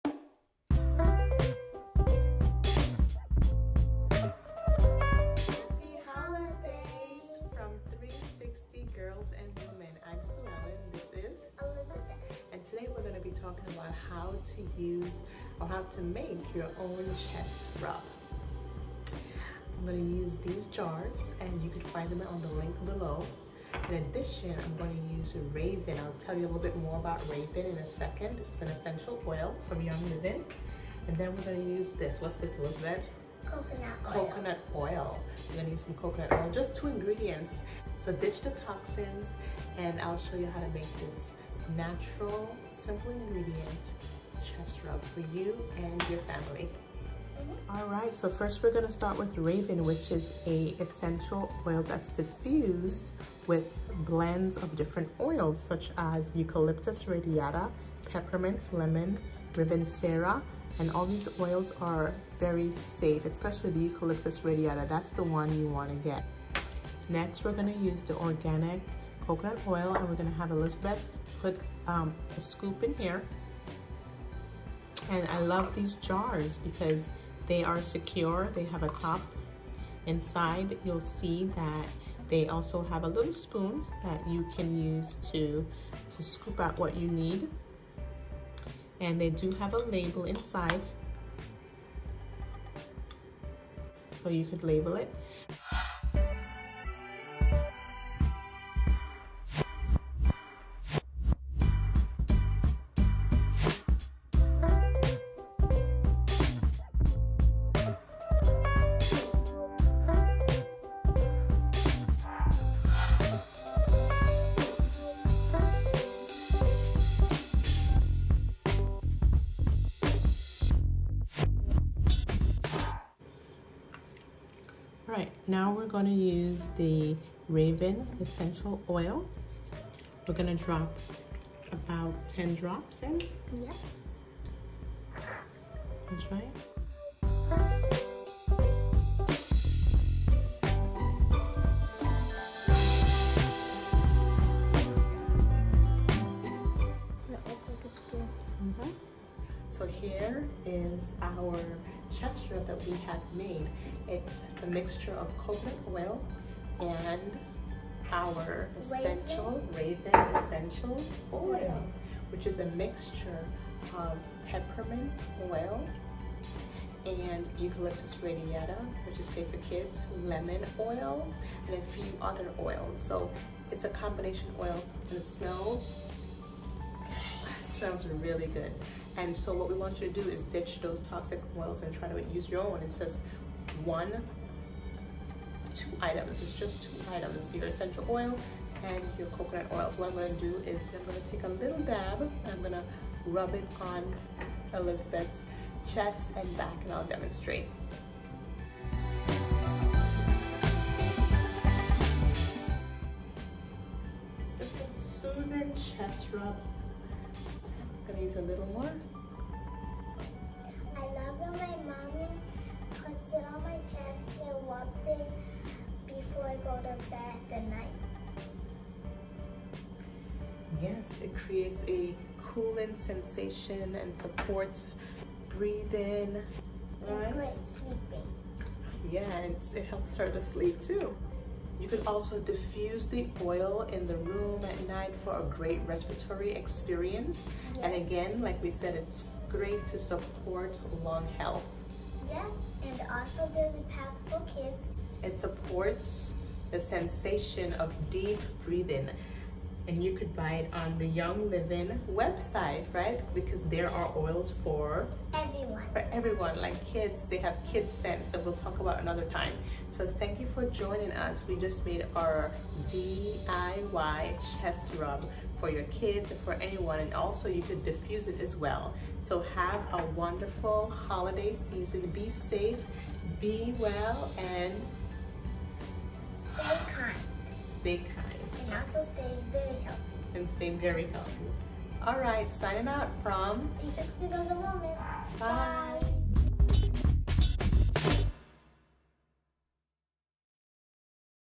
Two ingredient nontoxic chest rub
Winter is around the corner and we’re still in the midst of a pandemic. Let’s remember to take care of self and our family. Watch this video to see our two ingredient chest rub made WITHOUT harmful ingredients.

Links:
2 OZ ROUND GLASS JARS (12 PACK)